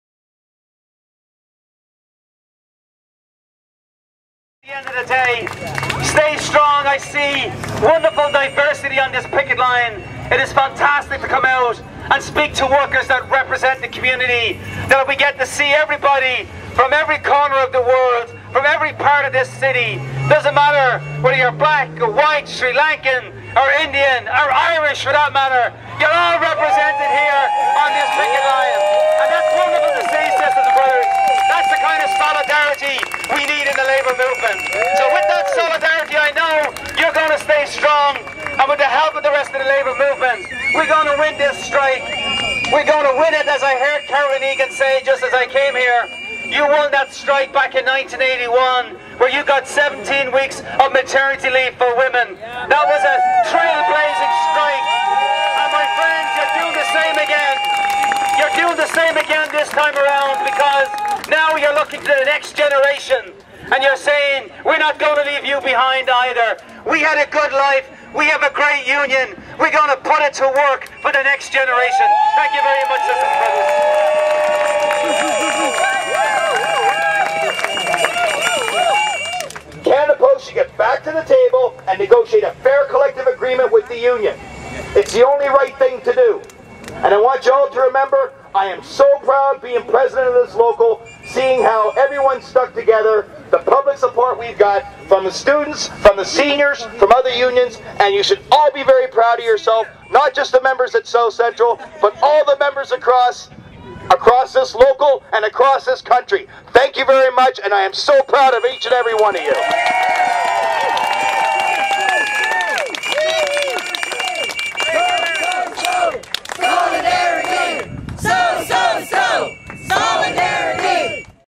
CUPW solidarity rally, june 14 2011
OFL President Sid Ryan and CUPW Toronto local president Darrell Ellis speak at the rally for postal workers at the South Central sorting plant.